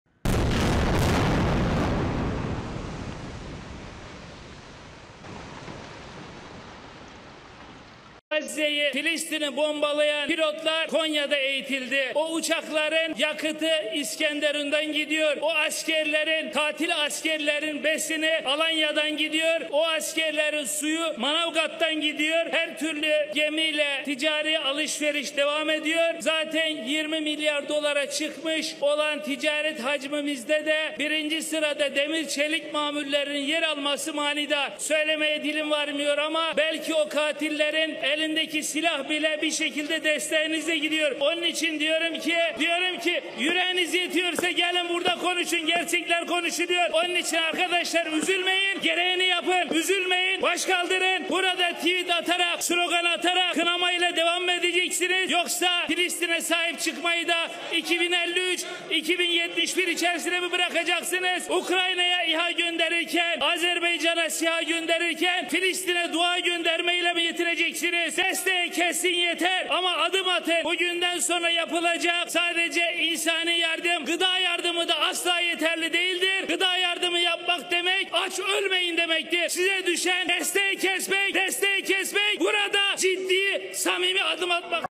Kontributi i qeverisë turke në bombardimin e Gazas
▪️Pilotët që kanë bombarduar Gazën dhe Palestinezët janë stërvitur në Konya(Turqi)!
▪️Karburanti i avionëve të tyre vjen nga Iskenderun (Turqi)!
▪️ Ushqimi për ushtarët e tyre vrasës vjen nga Alanya (Turqi)!
▪️Uji për ushtarët e tyre vjen nga Manavgati (lagje në provincën e Antalya)!

Tregtia detare e të gjitha llojeve vazhdon me ata! Ajo që bie në sy është se prodhimet e hekurit dhe çelikut zënë vendin e parë që arrin deri në 20 miliard dollar!

Ndoshta këtë që do ta them, s’do mund ta them, por madje edhe armatimi që është në duart e atyre vrasësve vjen nga financimi juaj.

Andaj unë them: nëse ju keni burrëri, hajdeni e flisni këtu! E vërteta do të thuhet! Prandaj, mos u pikëlloni o ju shokë! Bëni atë që duhet bërë! Mos u pikëlloni! Rebelohuni!

 Vazhdoni të postoni tweete, të postoni slogane, vazhdoni të qortoni, apo mos ndoshta edhe shpëtimin e Palestinës do e lini për në vitet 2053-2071?

Ndërkohë që për Ukrainën dërgonit drone, ndërkohë që për Azerbajxhanin dërgonit drone, e tani për Palestinën do mjaftoheni vetëm me lutje?!

Ndërprerja e mbështetjes (për Izarelin) do mjaftonte. Por ato ndihma humanitare dhe ushqimore që do të ofrohen nga sot e tutje (për Palestinën), nuk janë kurrë të mjaftueshme. Ndihma ushqimore është njësoj sikur te thuash “mos vdisni te uritur”. Ajo që ju takon juve është të ndërprisni mbështetjen tuaj (për Izraelin), të ndërprisni mbështetjen tuaj dhe të hidhni një hap serioz.